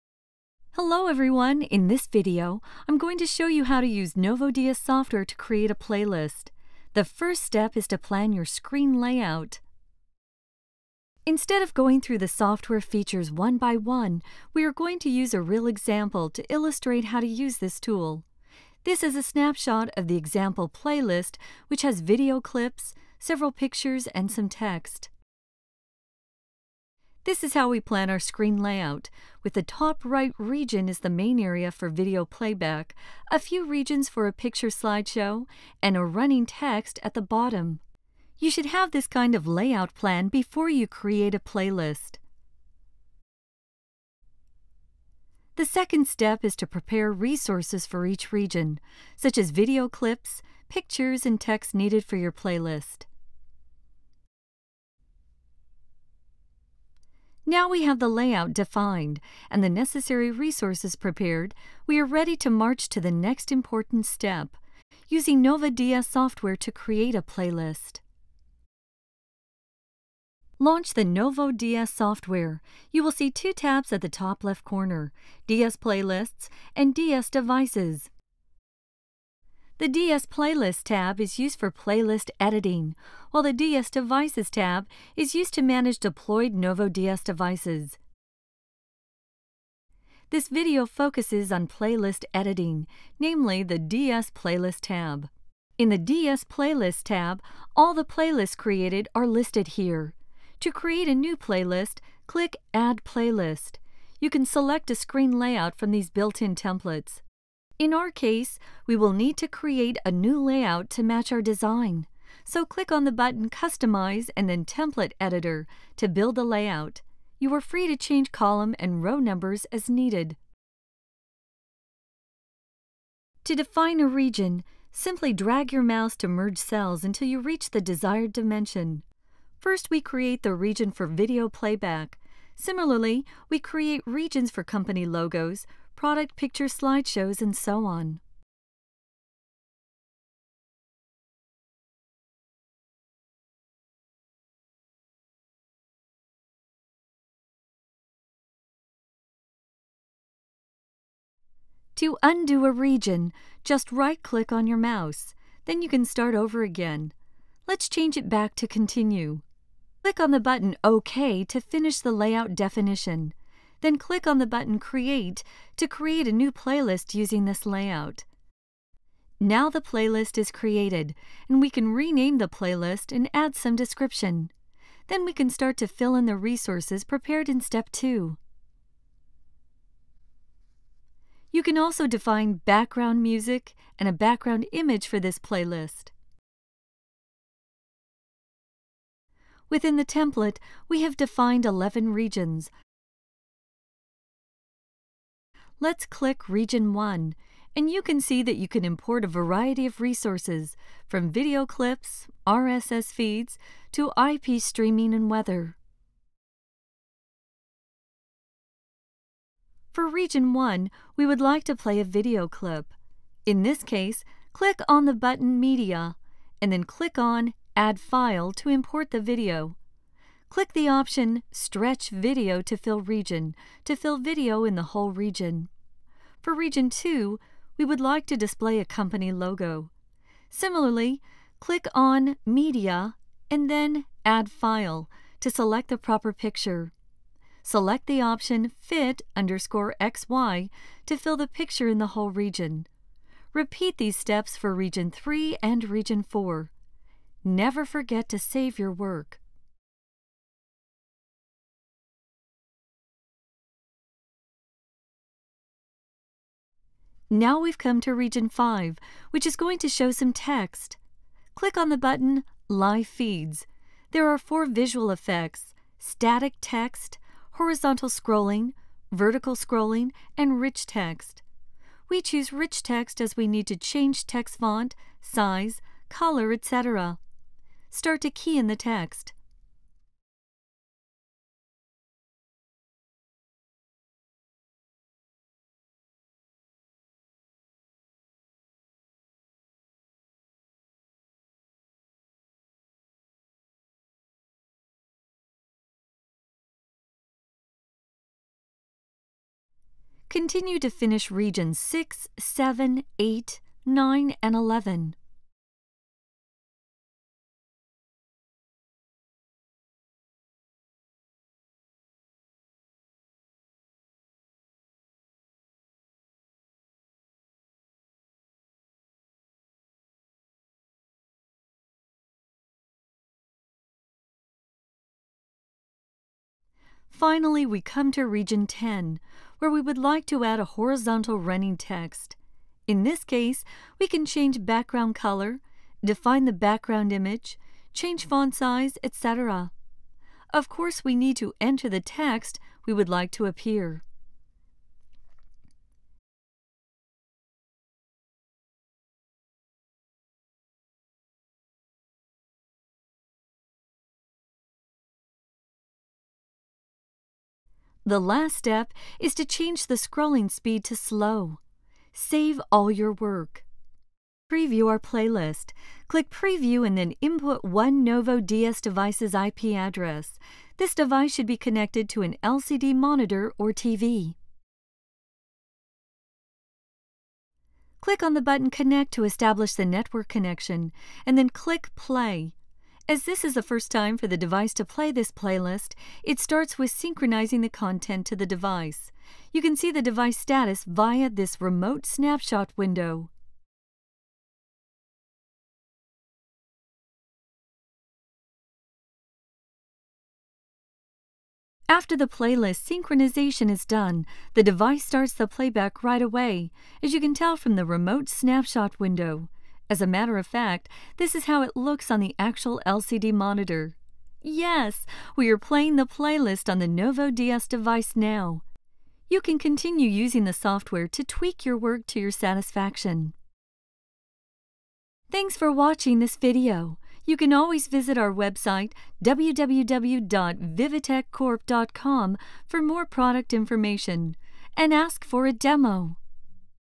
How to create a playlist using NovoDS digital signage solution software
Let’s learn how to create a playlist using NovoDS software.

For details about our digital signage solutions, please visit us